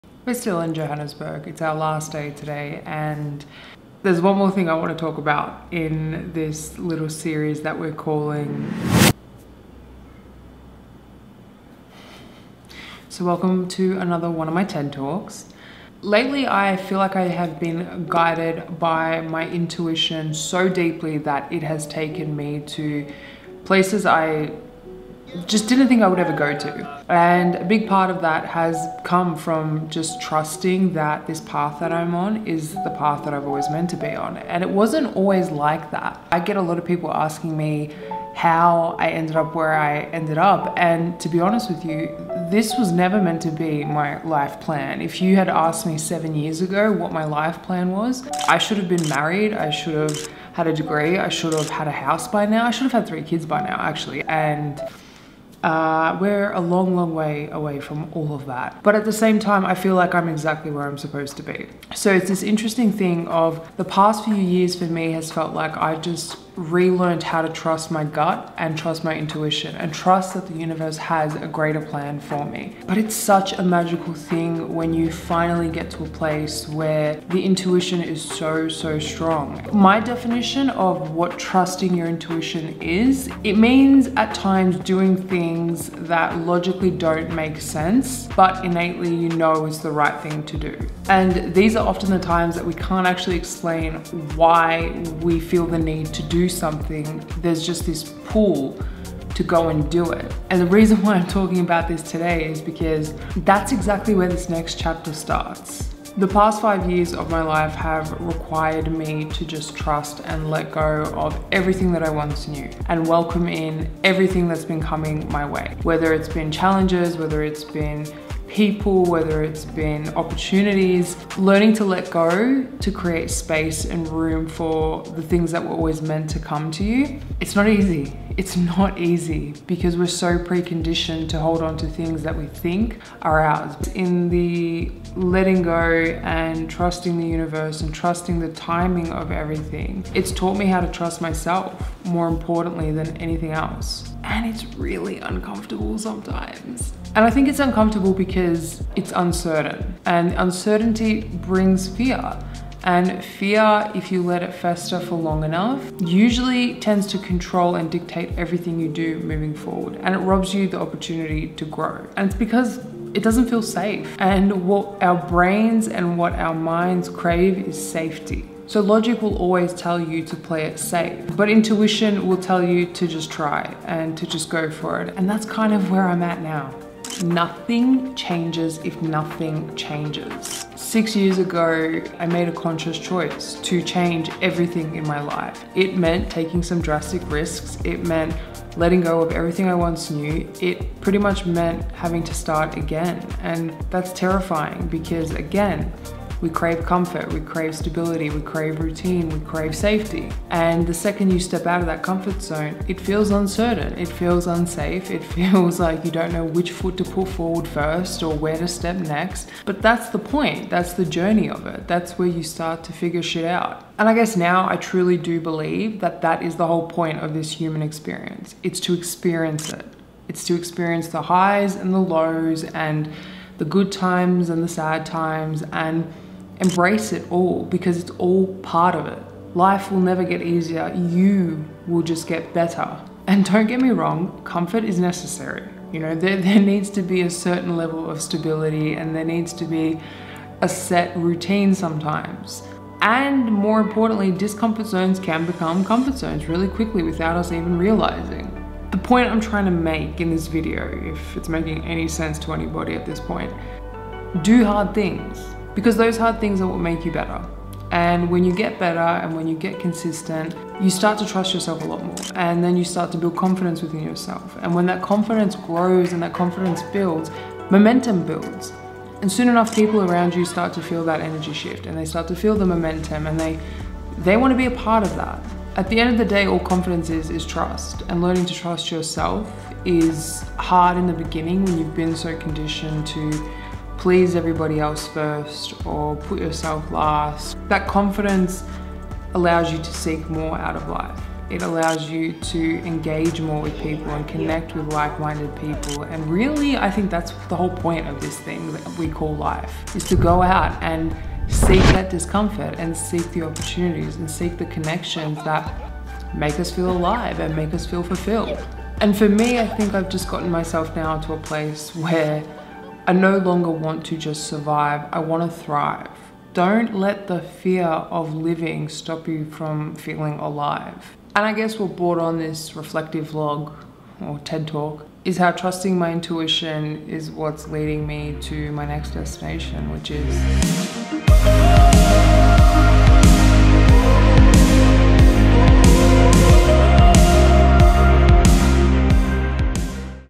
Trusting my intuition is leading me somewhere I never thought I'd go...
Last day in Johannesburg until I'm off to my next destination, a place I didn't think I would find myself on this trip. But when you learn to trust your intuition more than logic and the noise around you, it will take you to places you never dreamed you'd end up. And that's the journey I have been on for the past year.

And lastly, thanks for being here :)